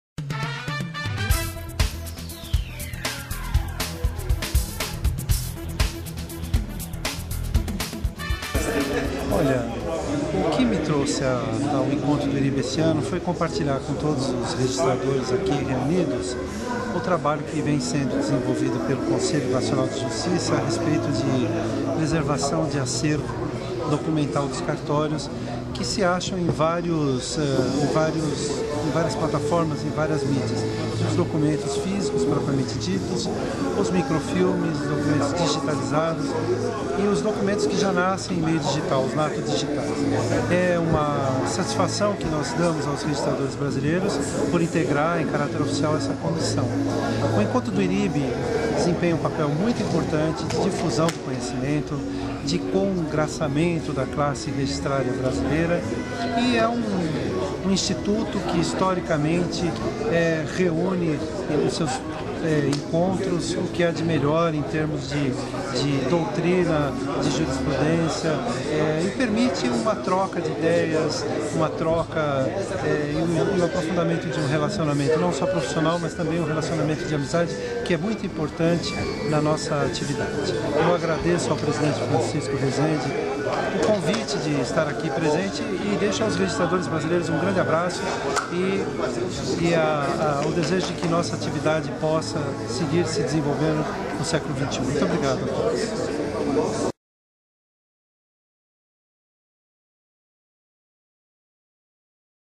Entrevista - Sérgio Jacomino, quinto oficial de registro de imóveis em São Paulo/SP
Entrevista com ex-presidente do IRIB concedida no 38º Encontro Nacional do IRIB em Fortaleza(CE).